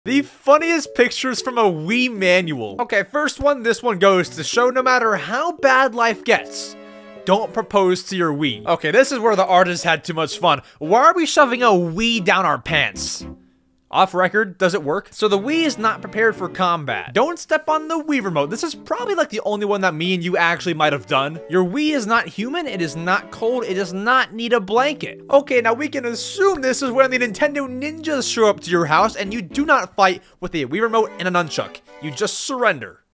The FUNNIEST Photos from a Wii Safety Manual... #2
🎮 Hey, there! Thanks so much for tuning in! Today, Max will be discussing the FUNNIEST photos from a Wii Safety Manual! Manuals are very cool and it's sad that we don't get them anymore, but man these manual photos from Japan are something else! Let's discuss! Sit back, relax, and enjoy the short! 🎮